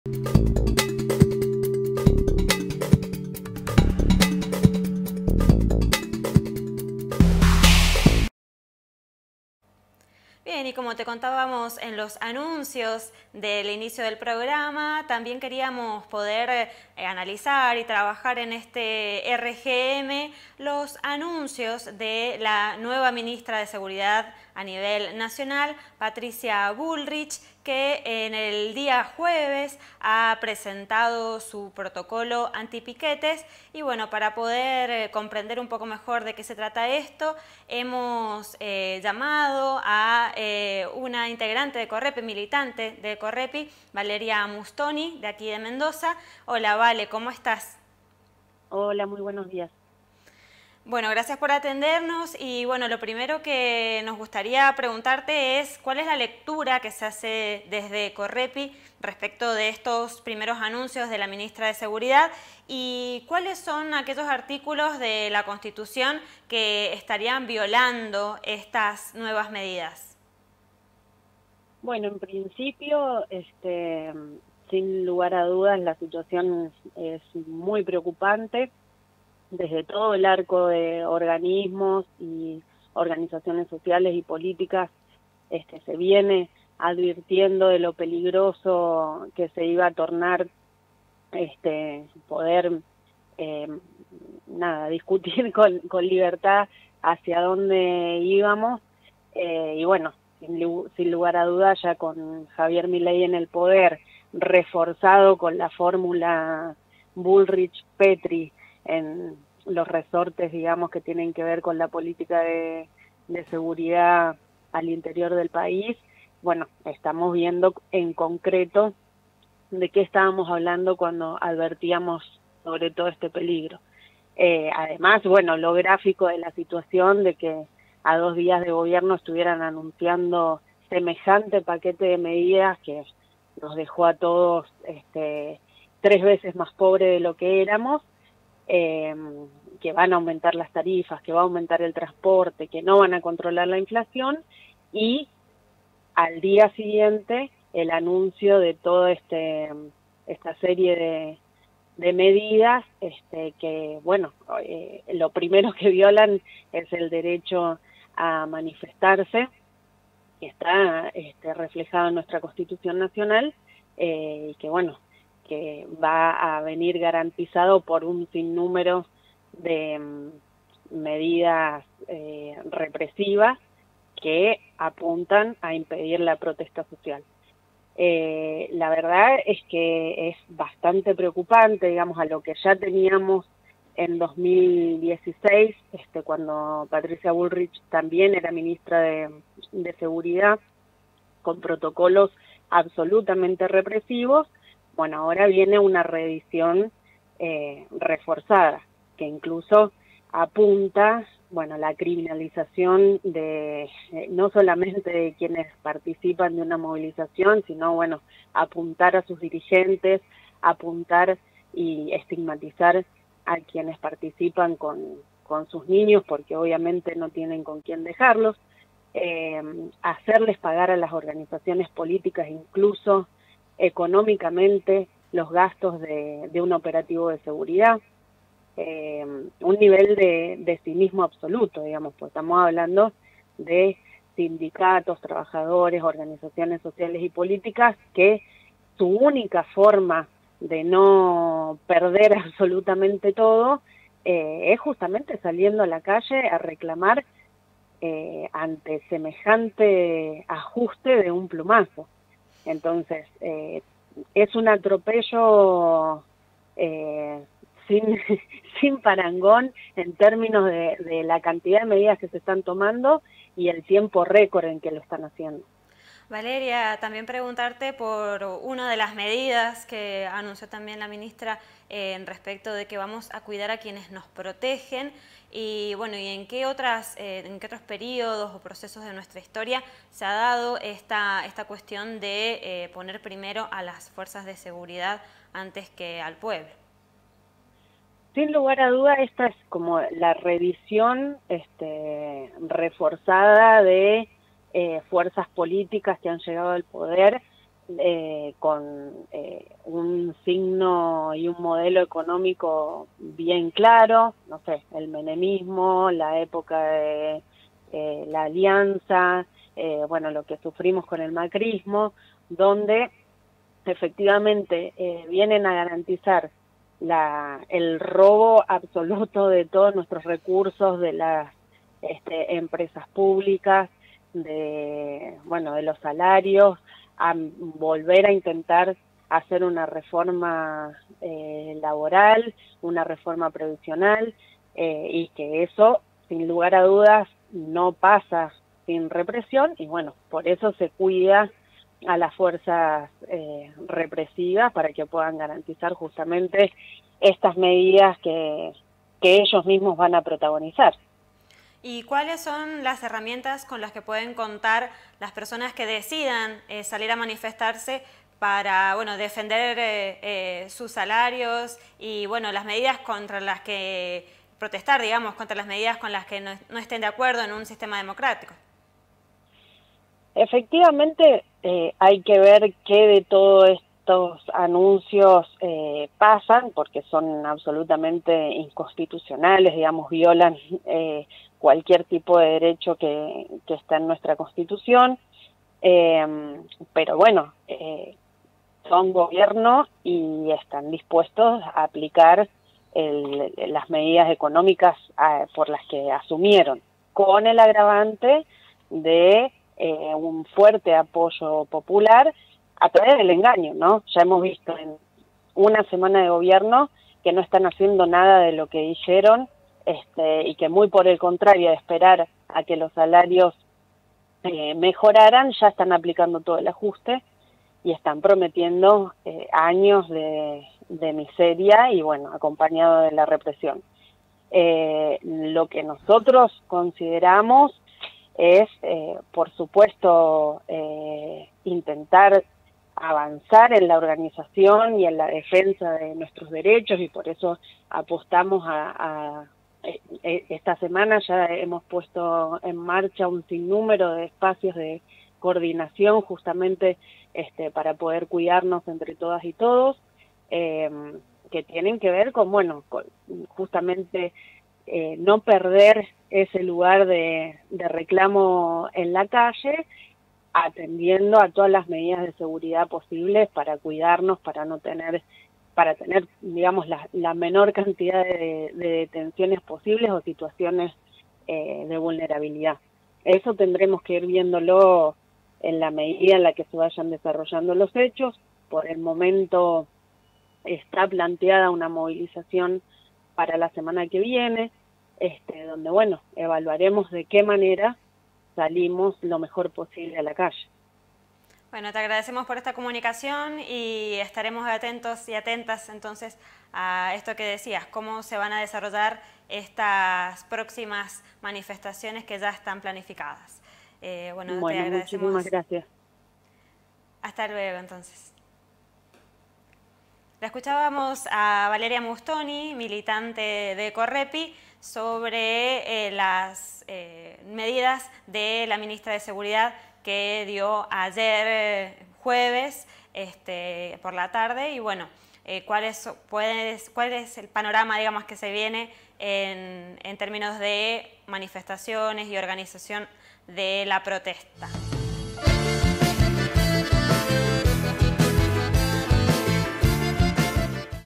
Respuesta de Correpi Mendoza ante los anuncios de Patricia Bullrich, actual Ministra de Seguridad
Este jueves 14 de diciembre Patricia Bullrich, ex- candidata a Presidenta por Juntos por el Cambio y actual Ministra de Seguridad de la Nación, anunció anunció un nuevo protocolo contra la protesta social. Lo hizo a días de los anuncios de ajuste económico realizados por Luis Caputo, ministro de Economía del gobierno de Javier Milei.
Entrevistamos a Valeria Mustoni, integrante de Correpi Mendoza que responde y reflexiona sobre lo peligroso de este nuevo protocolo para el pueblo, en el contexto socio-económico que atravies Argentina.